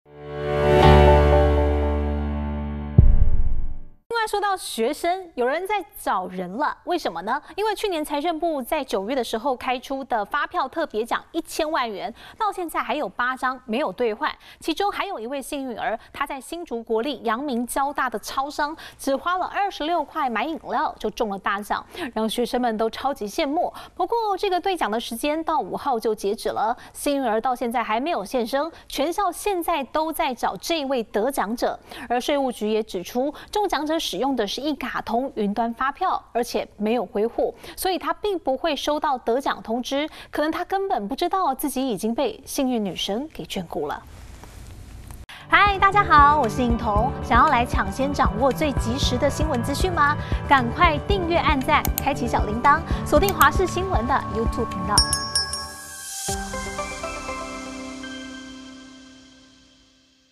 陽明交大開「千萬」大獎！ 稅務局：中獎者至今未領｜華視新聞 20240103
提醒你注意了！去年財政部在9月開出的發票特別獎1千萬元，至今還有8張沒有兌換，其中還有一位幸運兒，在新竹國立陽明交大的超商，只花26元買飲料就中了大獎，讓學生都超級羨慕，不過兌獎時間5日就截止，幸運兒至今都還沒現身，全校現在都在找這位得獎者！稅務局指出，中獎者使用的是一卡通雲端發票，而且「沒有歸戶」，所以並不會收到得獎通知，可能根本不知道自己已經被幸運女神眷顧。